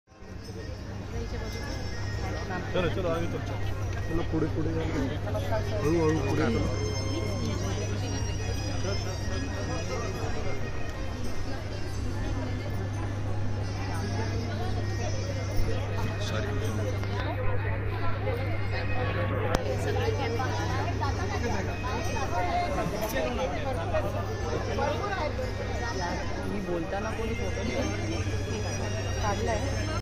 Union Minister Honble Smriti Irani ji & delegates of W20 visited our *HONEYWALA* Stall @G20/W20 meet
Union Minister Honble Smriti Irani ji , Union State Finance Minister Shri. Bhagwatji karad, shri Amitabhji Kant and delegates of W20 visited our Startup *HONEYWALA* Stall at G20/W20 meet held in Chhatrapati Sambhajinagar, (Aurangabad)Maharashtra,India. Thanks to support by MAGIC incubator...🙏